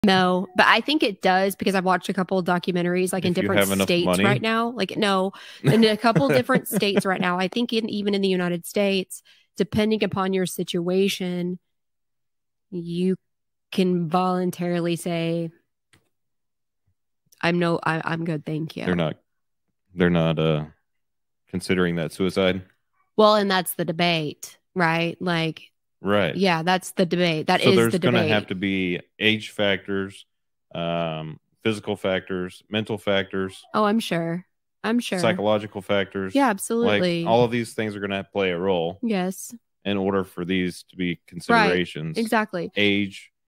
The Debate Over Euthanasia_ Age, Physical, Mental, and Psychological Factors
Should euthanasia be legal? It's a question that has sparked a heated debate, and one that has no easy answer.